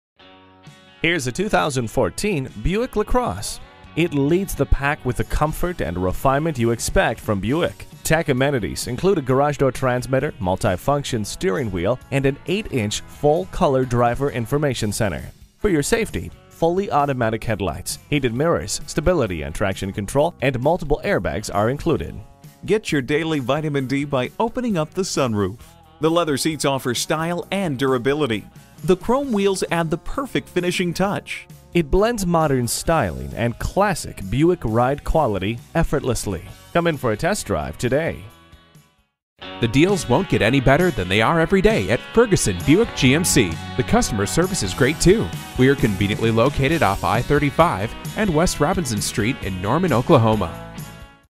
2014 BUICK LACROSSE PREMIUM Norman OK Oklahoma-City, OK B8034 - SOLD
Year: 2014
Make: BUICK
Model: LACROSSE PREMIUM
Engine: 3.6 Liter 6 Cylinder
Transmission: Automatic
Color: GBN
Interior: H1W LIGHT NEUTRAL/C
Stock #: B8034

Address:
1015 North Interstate Drive